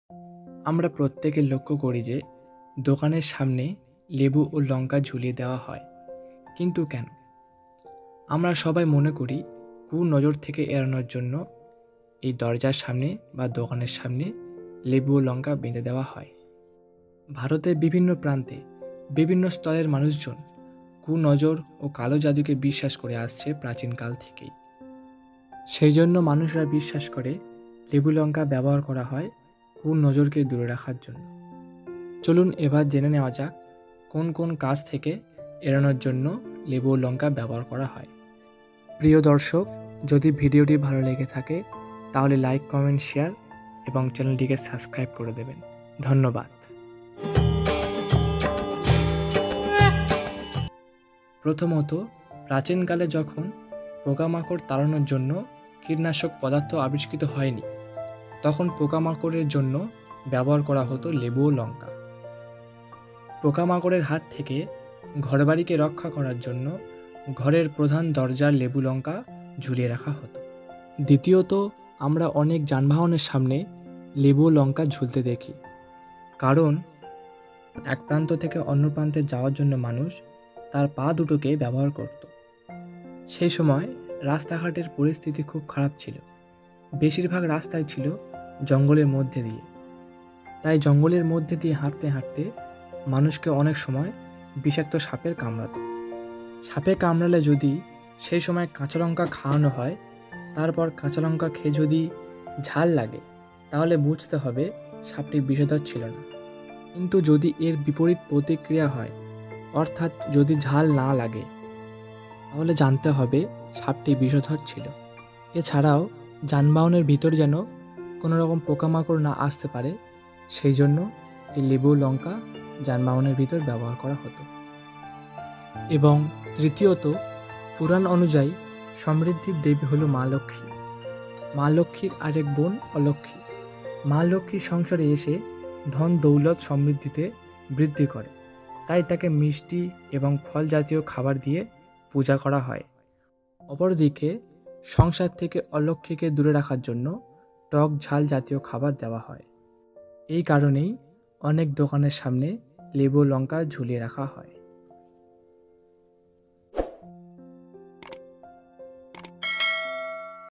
দোকানের সামনে লেবু ও লঙ্কা দেয়া হয় কেন ?
আমারা অনেকেই দেখি যে দোকান দরজায় সামনে  লেবু লঙ্কা ঝুলিয়ে রাখতে দেখা যায়। আমরা সবাই মনে করি কুনজর থেকে এড়ানোর জন্য এই দরজার সামনে লেবু লঙ্কা বেঁধে দেওয়া হয়। ভারতের বিভিন্ন প্রান্তের বিভিন্ন স্তরের মানুষজন কুনজর বা কালা জাদুকে বিশ্বাস করে আসছেন প্রাচীনকাল থেকেই। মানুষজন বিশ্বাস করেছি এই লেবু লঙ্কা ব্যবহার করা হয় কুনজরকে দূরে রাখার জন্য। চলুন এবার জেনে নেওয়া যাক কোন কোন কাজ থেকে এড়ানোর জন্য লেবু লঙ্কা ব্যবহার করা হয়।
১) যখন পোকামাকড় তাড়ানোর জন্য কীটনাশক পদার্থ আবিষ্কৃত হয়নি তখন পোকামাকড়ের জন্য ব্যবহার করা হতো লেবু এবং লঙ্কা। পোকামাকড়ের হাত থেকে ঘরবাড়ি কে রক্ষা করার জন্য ঘরের মেন দরজায় লেবু এবং লঙ্কা ঝুলিয়ে রাখা হতো।
2)আমারা অনেকেই দেখি যে যানবাহন সামনে  লেবু লঙ্কা ঝুলিয়ে রাখতে দেখা যায়। মানুষ যখন একপ্রান্ত থেকে অন্যপ্রান্তে যাওয়ার জন্য মানুষ তার পাদুটোকে ব্যবহার করত। তখনকার রাস্তাঘাটের পরিস্থিতি খুব খারাপ ছিল বেশিরভাগ রাস্তায় ছিল জঙ্গলের মধ্য দিয়ে তাই জঙ্গলের মধ্য দিয়ে হাঁটতে হাঁটতে মানুষকে অনেক সময় বিষাক্ত সাপের কামড়াতো।সাপে কামড়ালে যদি সেই সময় কাঁচালঙ্কা খাওয়া হয় তারপর কাঁচালঙ্কা খেয়ে যদি ঝাল লাগে তাহলে বুঝতে হবে সাপটি বিষধর ছিল না কিন্তু যদি এর বিপরীত প্রতিক্রিয়া হয় অর্থাত্‍ কেউ যদি না লাগে তাহলে জানতে হবে সাপটি বিষধর ছিল।
৩) এছাড়াও পুরাণ অনুযায়ী সমৃদ্ধি দেবী হল মা লক্ষ্মী। লক্ষীর আরেক বোন অলক্ষী। মা লক্ষ্মী সংসারে এসে ধন-দৌলত সমৃদ্ধিতে বৃদ্ধি করে তাই তাকে মিষ্টি এবং ফল জাতীয় খাবার দিয়ে পূজা করা হয়। অপরদিকে সংসার থেকে অলক্ষী কে দূরে রাখার জন্য টক ঝাল জাতীয় খাবার দেওয়া হয়। এই কারণেই অনেক বাড়ি বা দোকানের সামনে লেবু লঙ্কা ঝুলিয়ে রাখে।